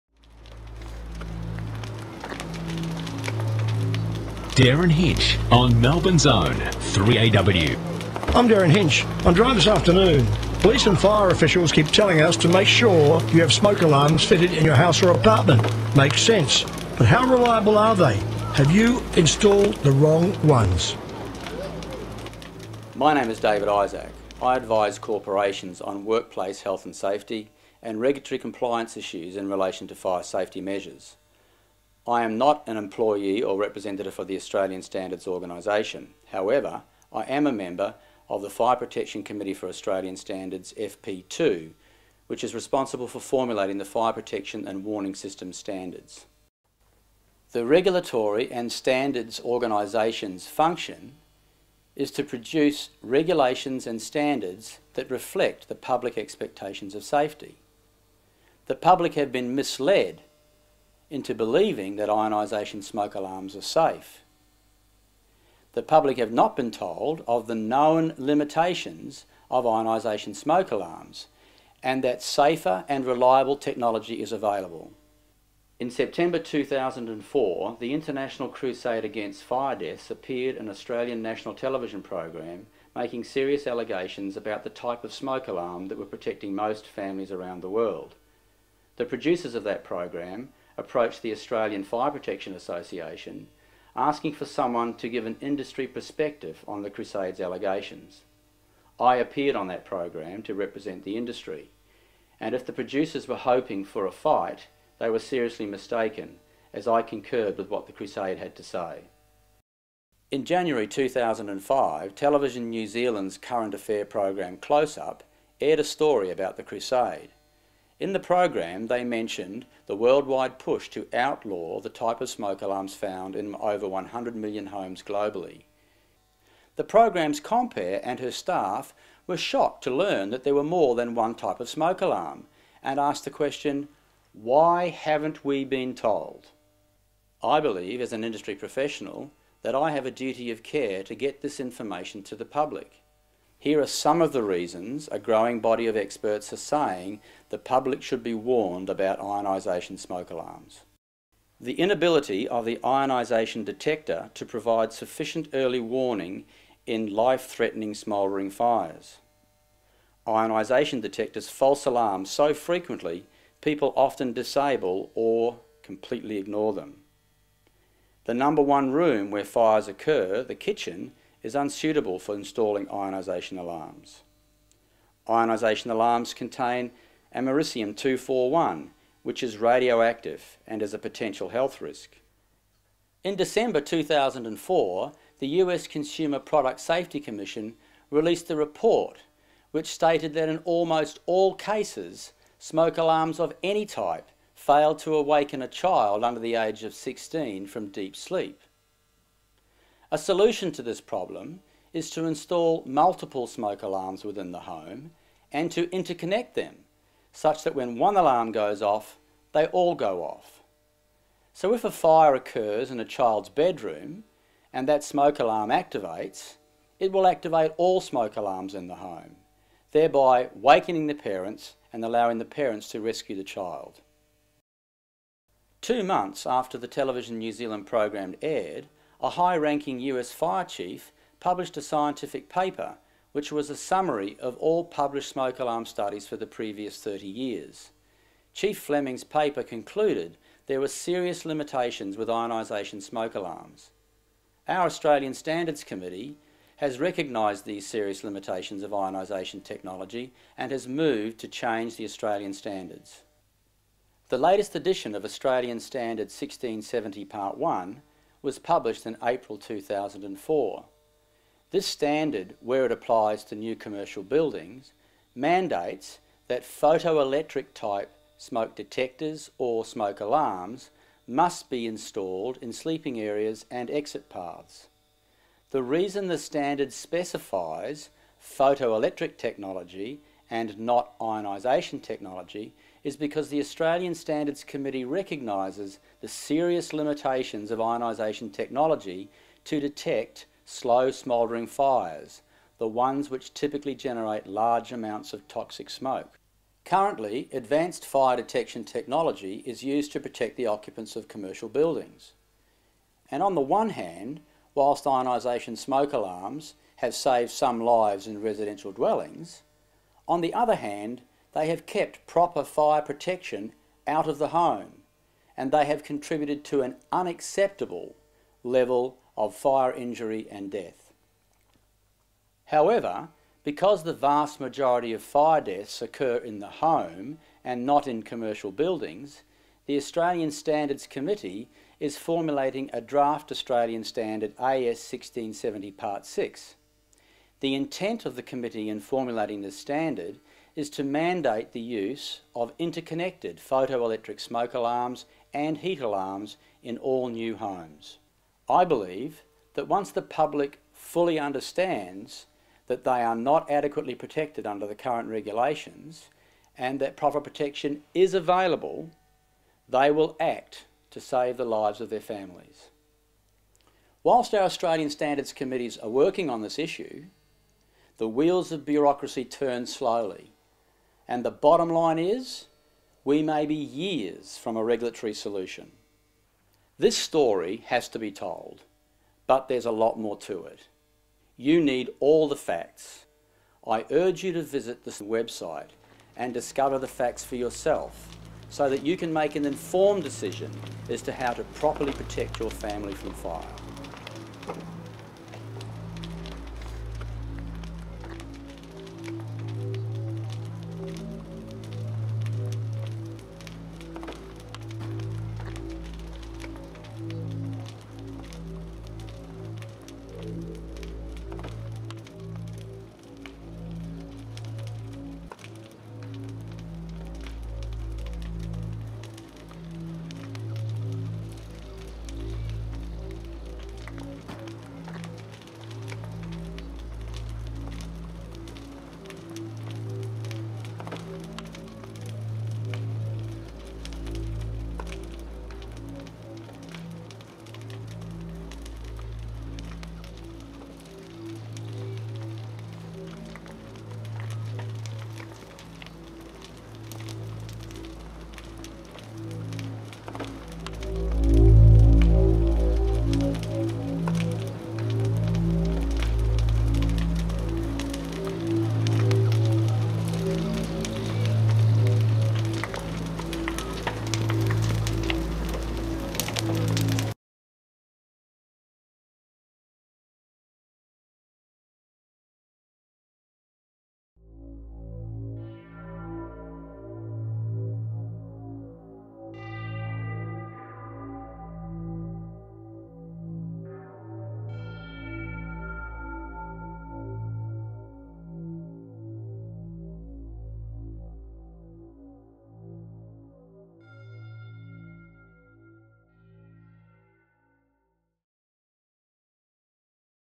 Ionization Smoke Alarms are DEADLY | v2.8
There are two types of smoke alarms, ionization and photoelectric (both types can be either battery or hard-wired).
If you have smoke alarms in your home chances are more than 95% they are the ionization type. These alarms are DEADLY because they do not safely detect fire in the early, smouldering stage.
FLAWED STANDARD
A dangerous loophole in Australia's smoke alarm standard has allowed ionization alarms to be sold since 1993. Because of the deadly limitations of ionization smoke alarms, photoelectrics have been mandatory in sleeping areas and exit paths in new commercial buildings since 2004.
After the film, help protect your family and expose the truth.  Allow the World Fire Safety Foundation to send a 'Smoke Alarm Disclosure Letter' (it's free) by registered mail to YOUR smoke alarm manufacturer.  Discover the facts about the DEADLY ionization smoke alarms in your home:
Filmed: Aug, 2006 - Updated 28 April, 2011 | Version: V2.8